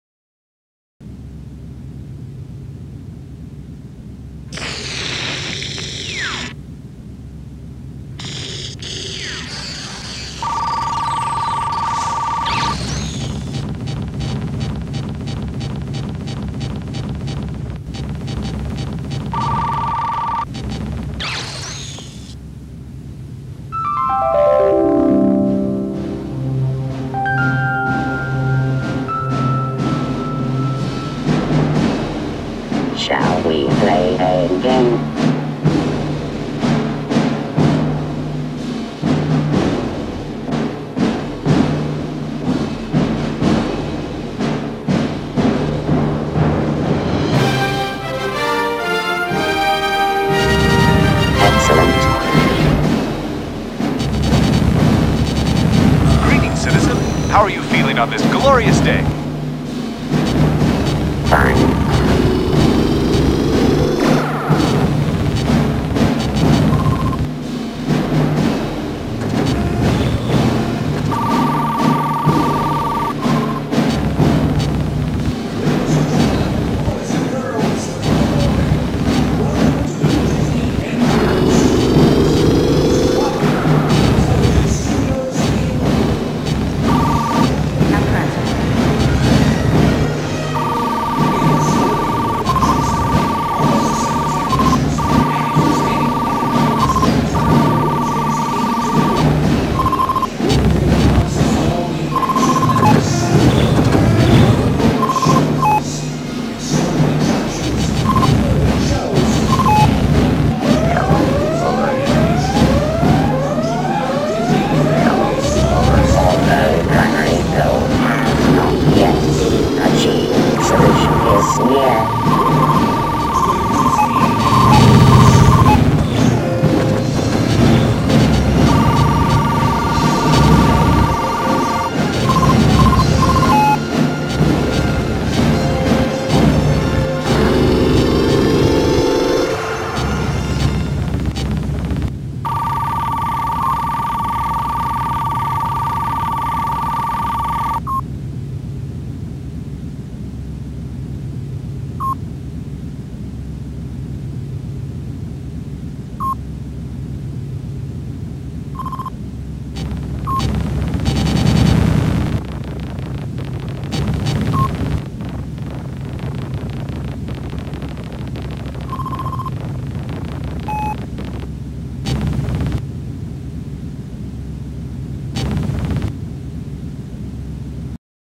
Disnet
Simulation of a fictional internet service whose only purpose is to provide harmony in DisneyWorld Themeparks whenever there is disorder.  2000.